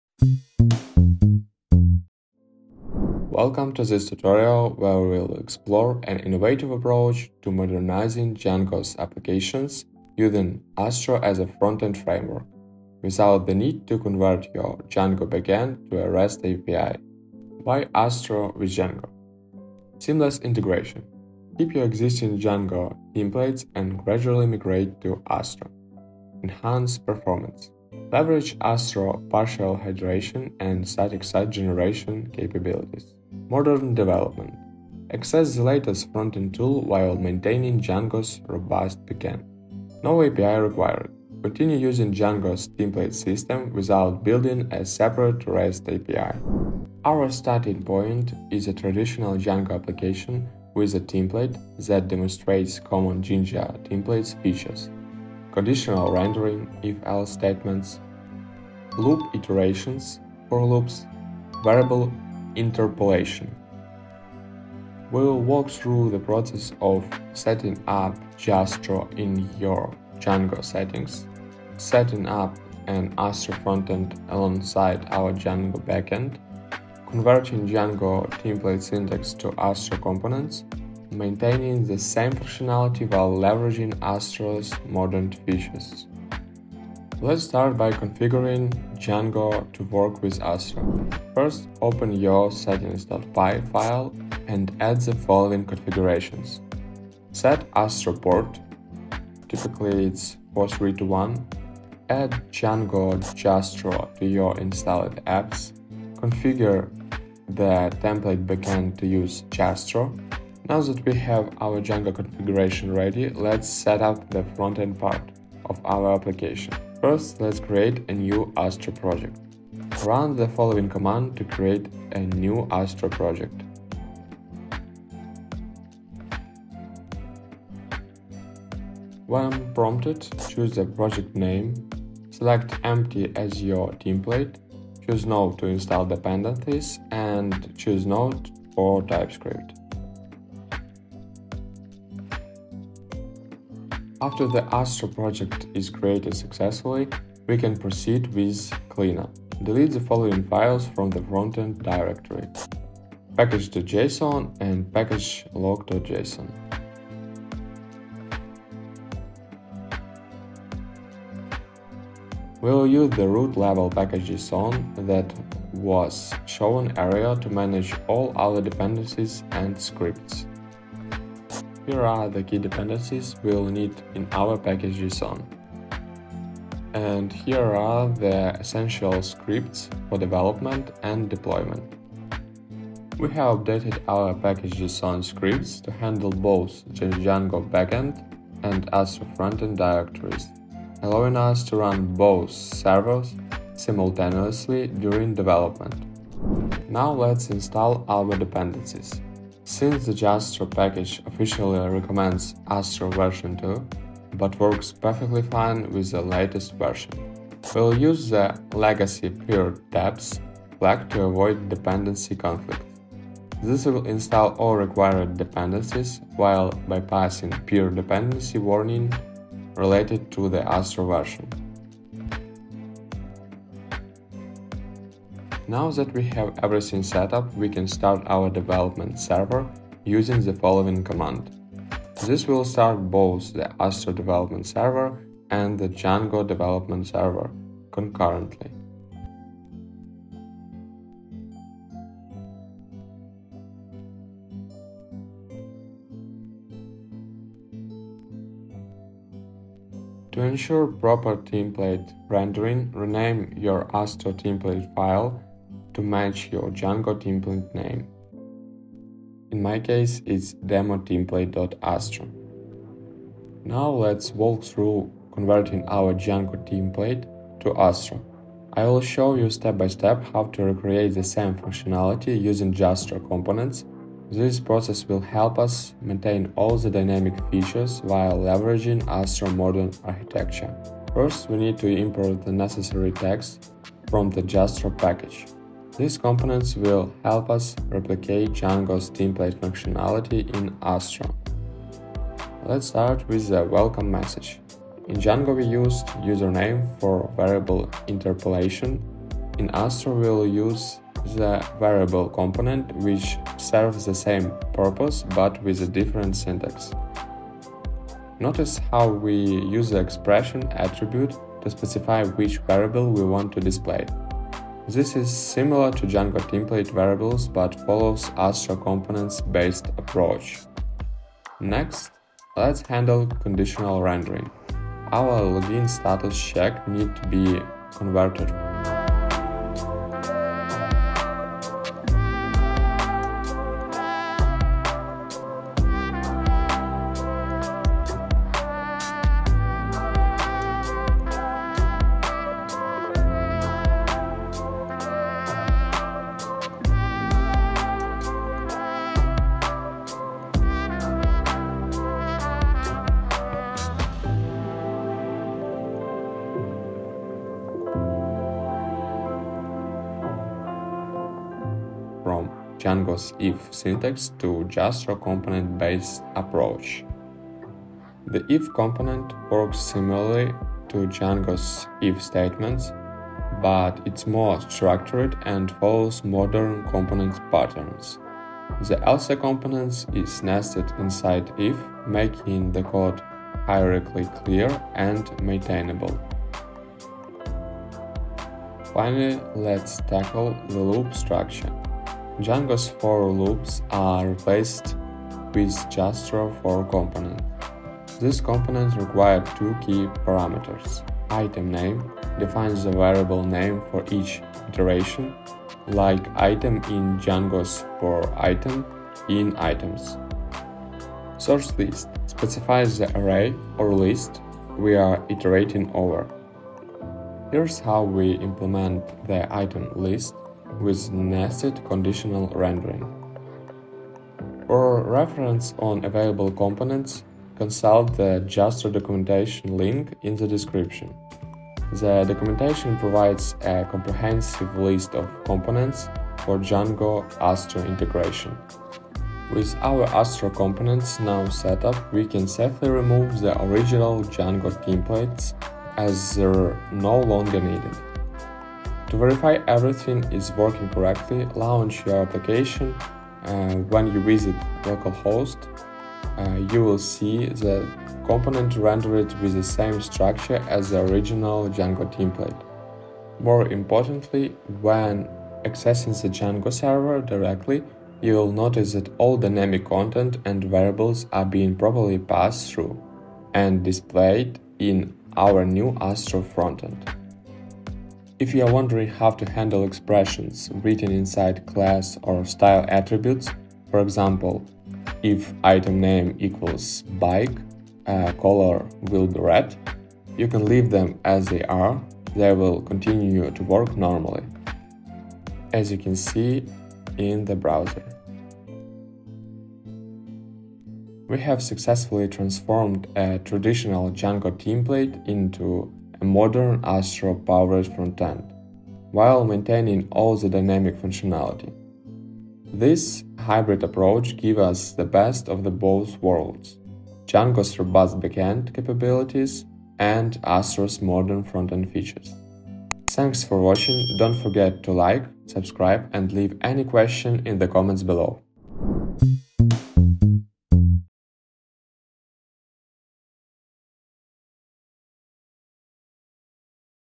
Djazztro Magic: Combining Django Backend with Astro Frontend
🚀 Modernize Your Django Frontend with Astro! Learn how to seamlessly integrate Astro with Django without converting your backend to a REST API. In this tutorial, we’ll guide you through:

    Setting up Djazztro to connect Django with Astro
    Converting Django templates to Astro components
    Running Django and Astro side by side for the best of both worlds

🌐 Book a free 30 minute call with our expert Django developers